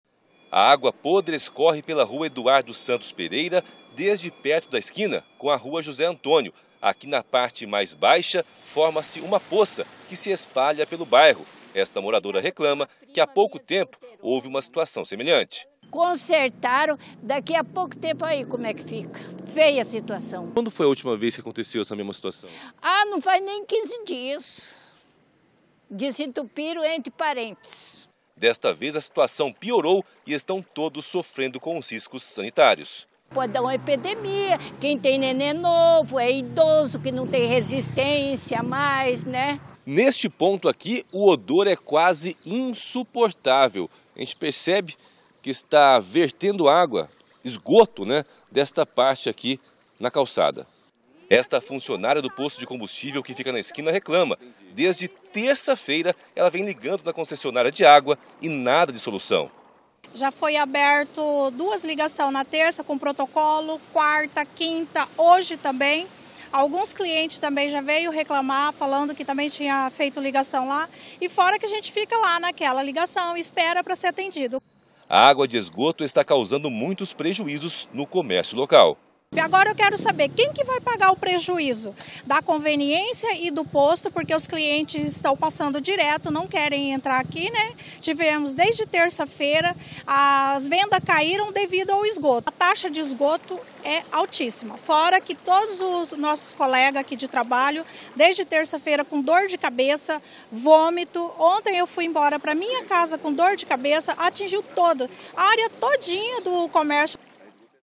Moradores reclamam de vazamento de esgoto no centro de Campo Grande
Moradores e comerciantes próximos á área central de Campo Grande estão indignados com um vazamento de esgoto. O problema já está acontecendo desde terça-feira (10).